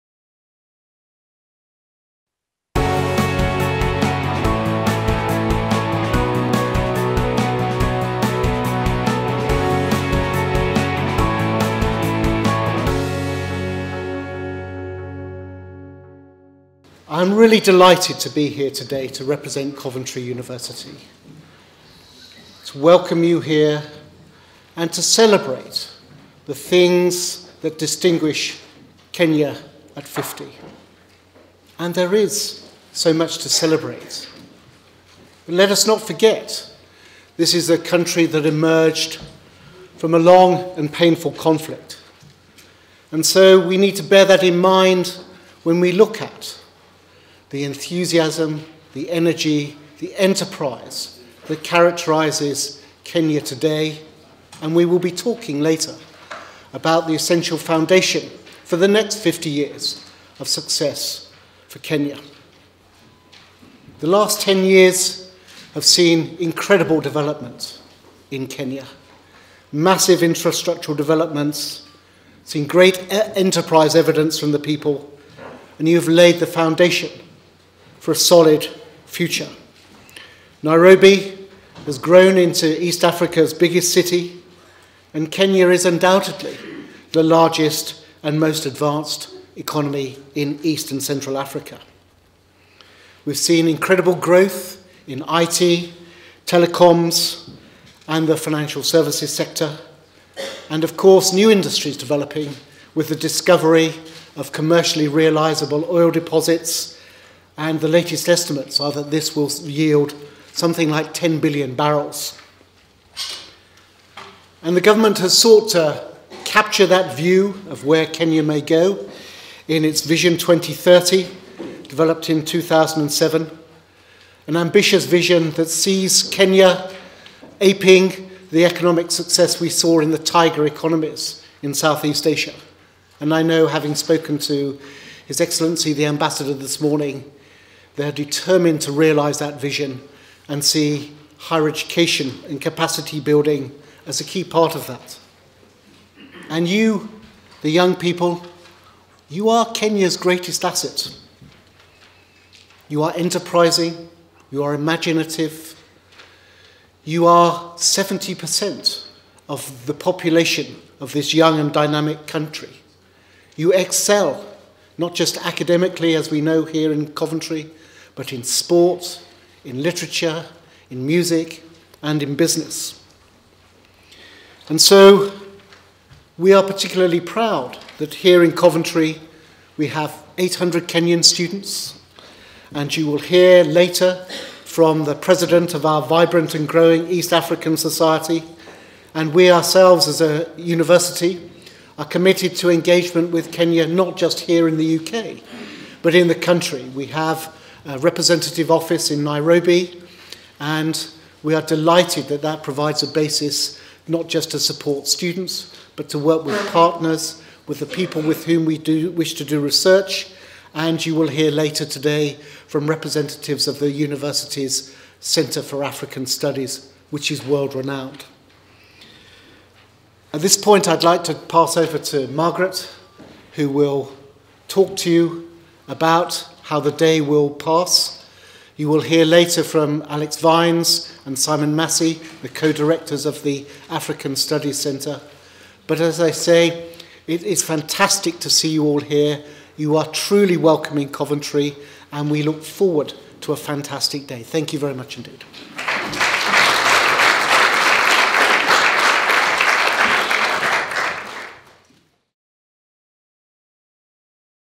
KENYAN STUDENTS' CONFERENCE Pro Vice Chancellor speech
Kenyan University Student in UK had a successful conference marking Kenya @ 50. This is the first time an event of this kind has taken place .The event took place at Coventry University which has seen over 800 students pass through the University within the years . The Pro Vice Chancellor praised the Kenya Government for laying the essential foundation in the education sector. The event was organise through the education department at the Kenya High Commission headed by the Education Counsellor , Mrs  Margaret Lessuda,
It was also attended by other university students from UK